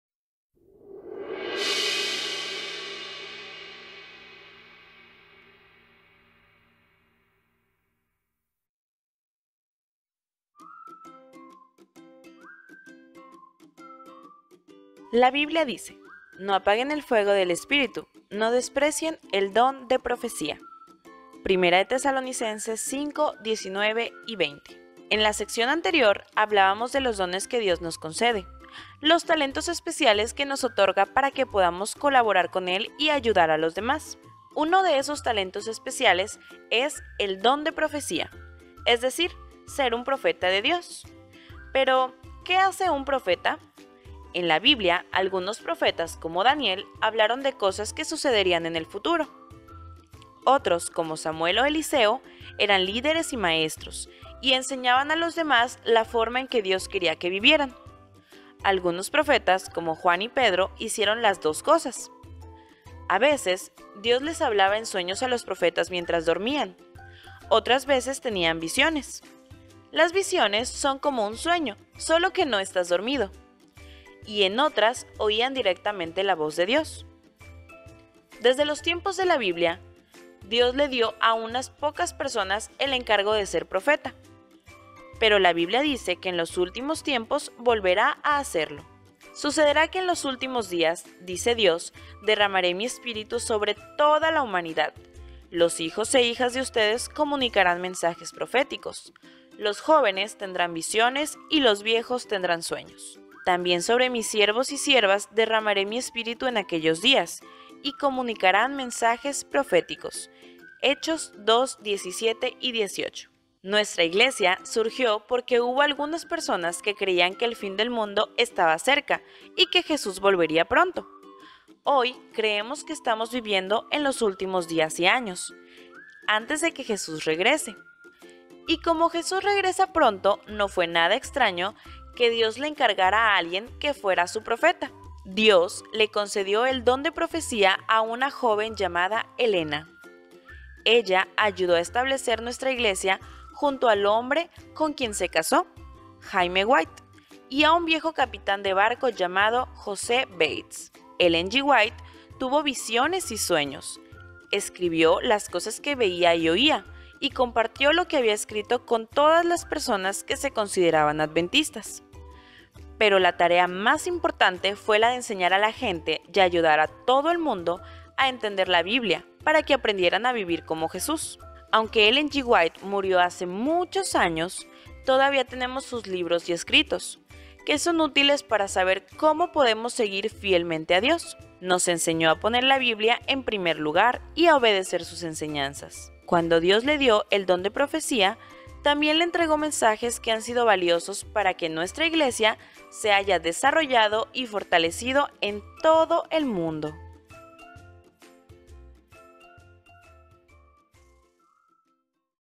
18. El don de profecía (28 Creencias Adventistas Para Niños)
Descubre quién fue una mensajera de Dios.